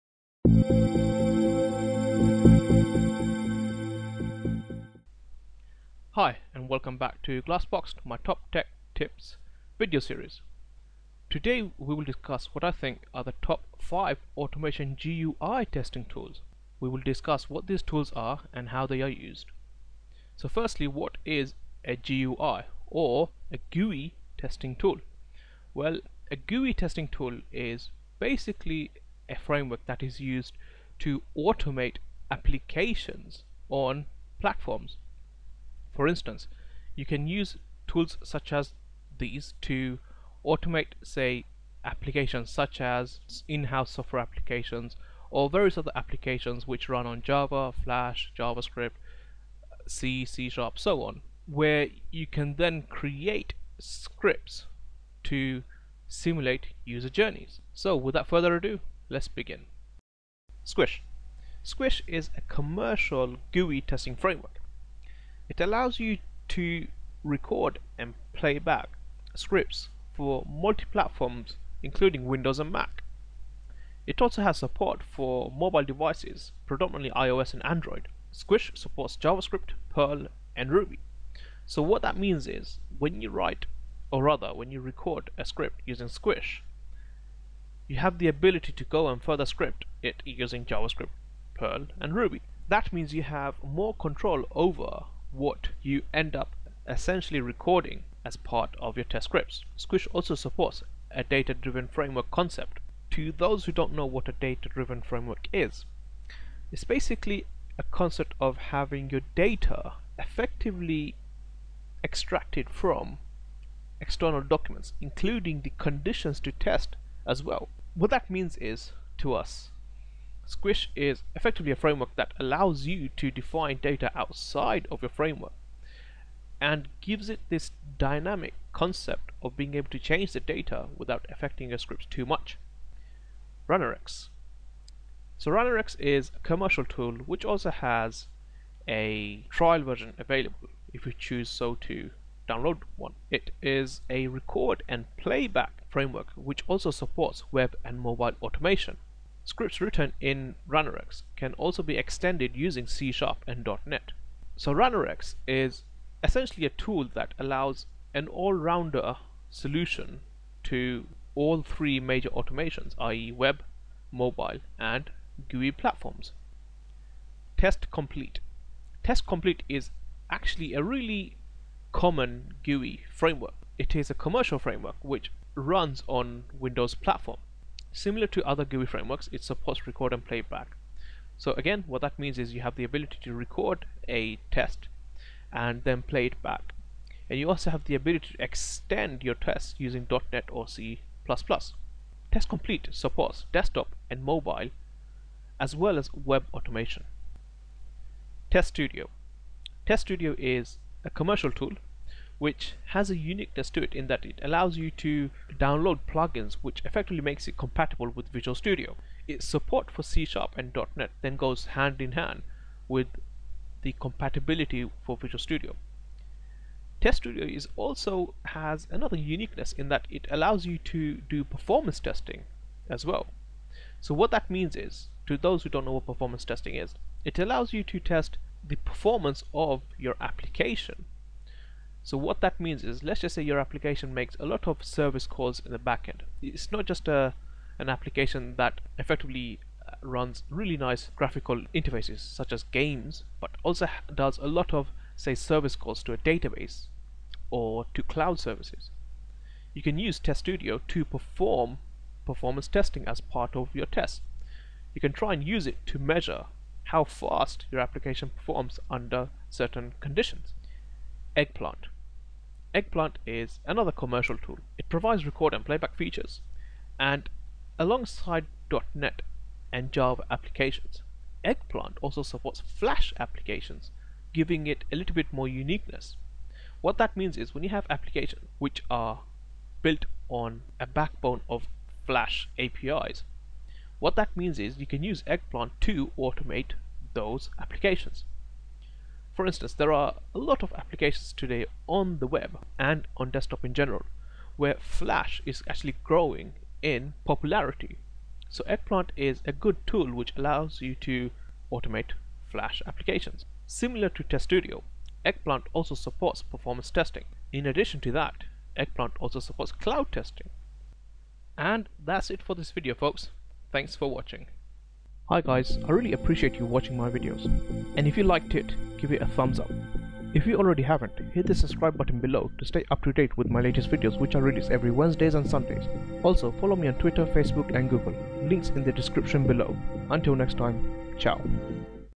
Top Tech 07 (GUI Automation Tools) | QAShahin
Top Tech video on the Top Five GUI Automation Tools available.

Resources:
Introductory music: